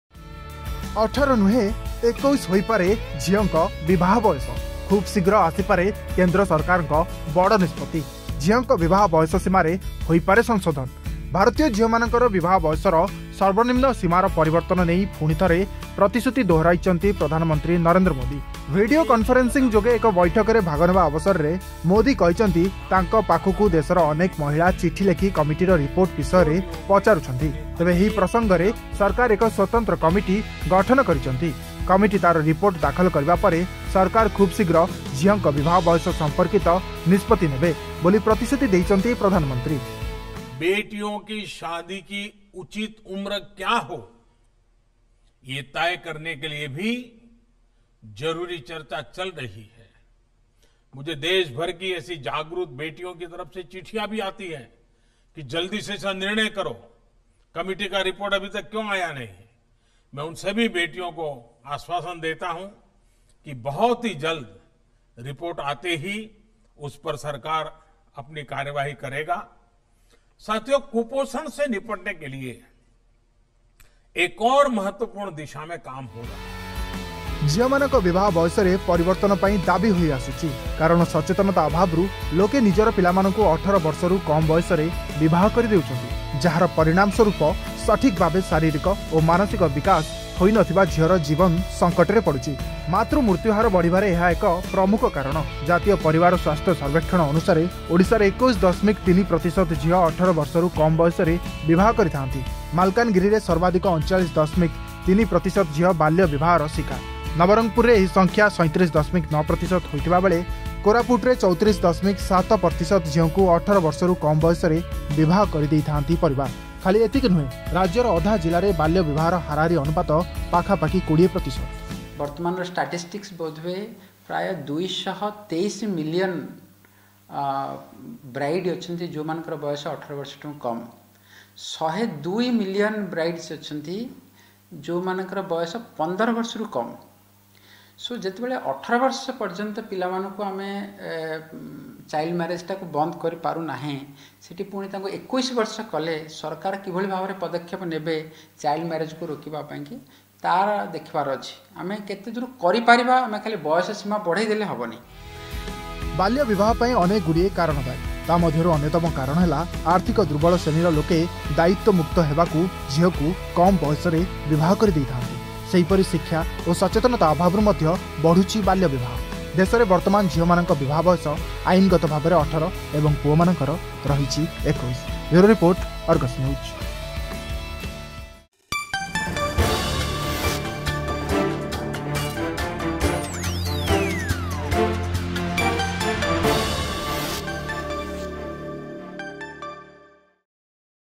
18 ନୁହେଁ 21 ହୋଇପାରେ ଝିଅଙ୍କ ବିବାହ ବୟସ
18 ନୁହେଁ 21 ହୋଇପାରେ ଝିଅଙ୍କ ବିବାହ ବୟସ । ଏନେଇ ଖୁବ ଶୀଘ୍ର ଆସିପାରେ କେନ୍ଦ୍ରର ନିଷ୍ପତ୍ତି.